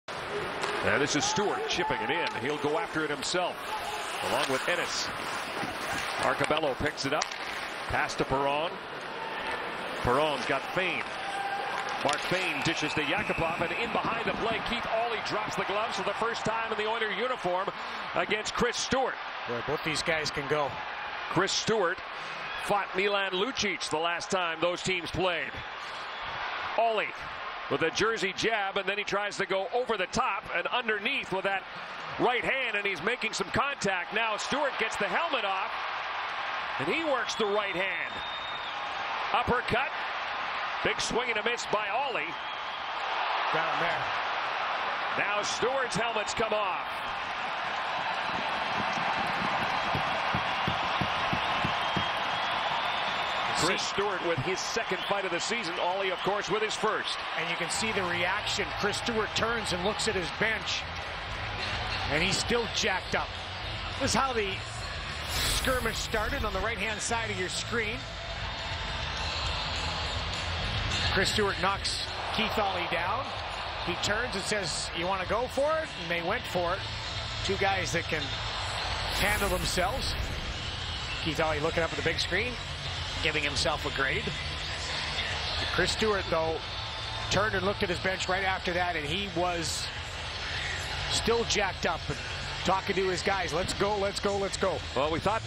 Keith Aulie vs Chris Stewart Nov 7, 2014
Keith Aulie vs Chris Stewart from the Edmonton Oilers at Buffalo Sabres game on Nov 7, 2014.